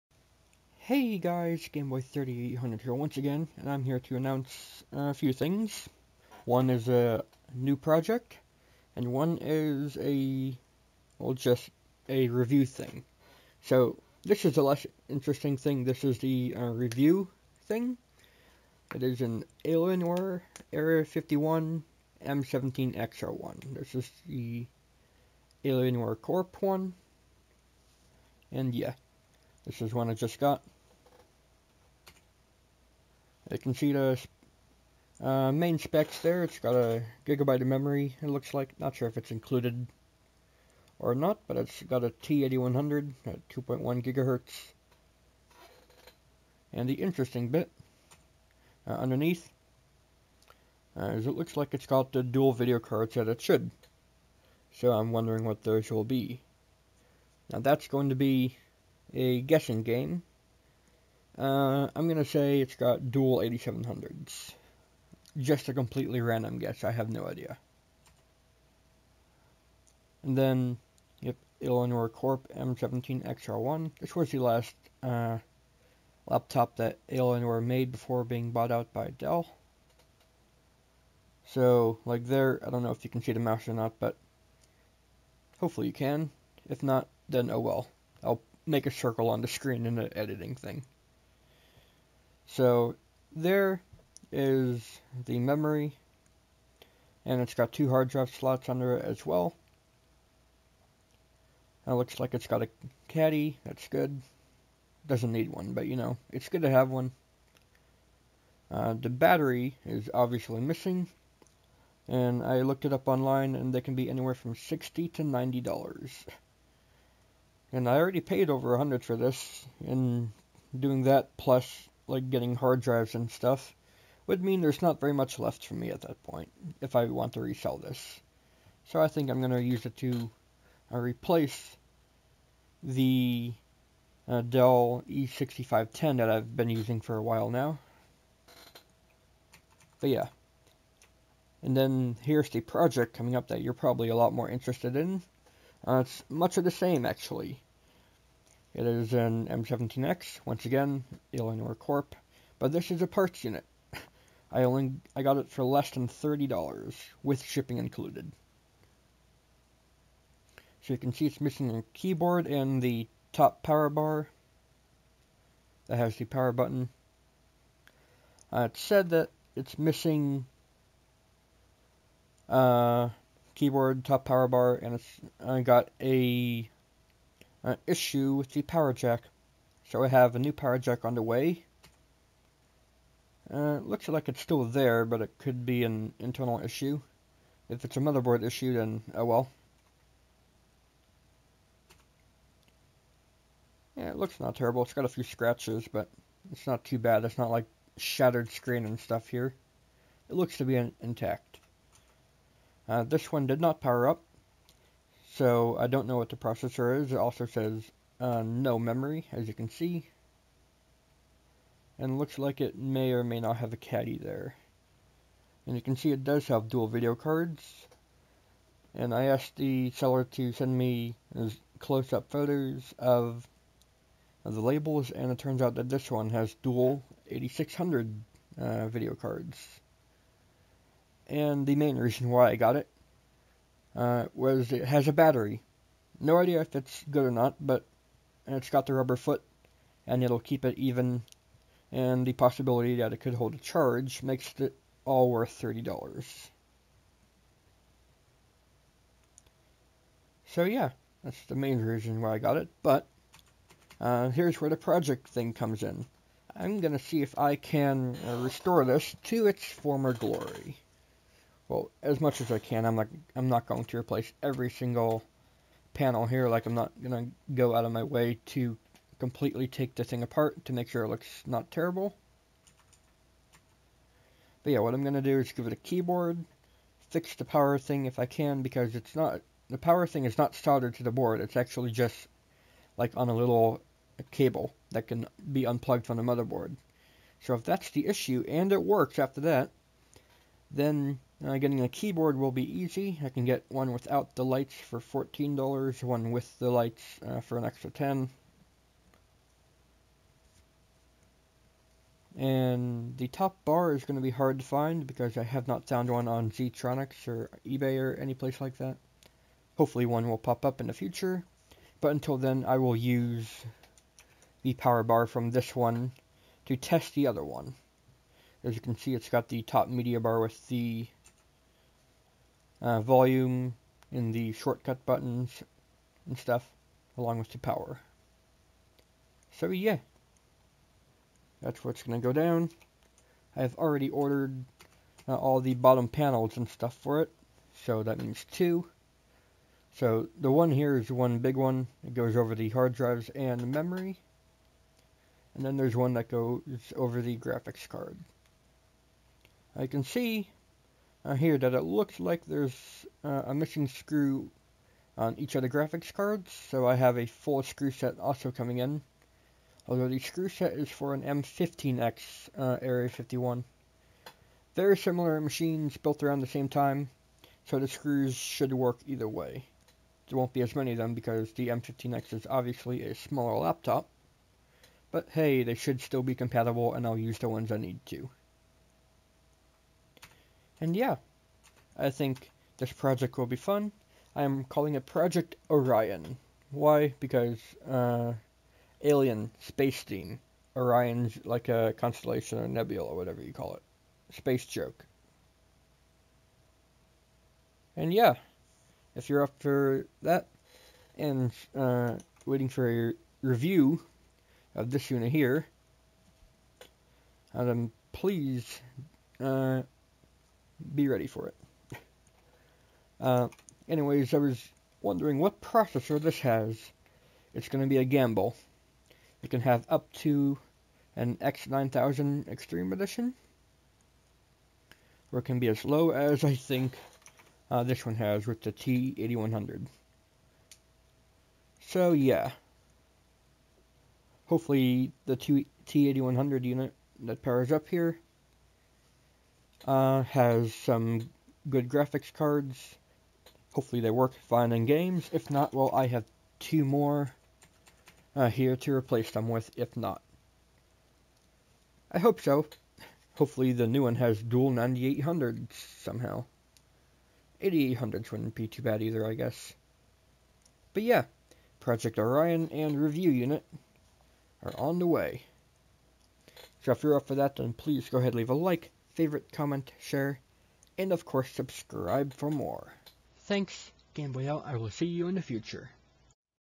Project Orion announcement + more!
2 similar projects. one of these is for review purposes, so not many vids need to be made of it (one for its arrival, one for os install, then review. if any issues pop up then expect 1 or 2 more. but not much else). the other is the actual Project Orion, which should have a lot more vids to it (arrival, 1 - 3 part arrival/install ones, 1 - 3 testing ones, os install, finish, and maybe 1 or 2 more afterwards).
Project Orion i think will be really interesting because i never restored a laptop on video before. and i think it'll be fun. let me know what you think in the comments.